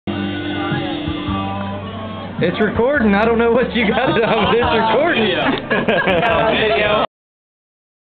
Dinner at the spec 120508
Waiter accidently videoed instead of taking a picture.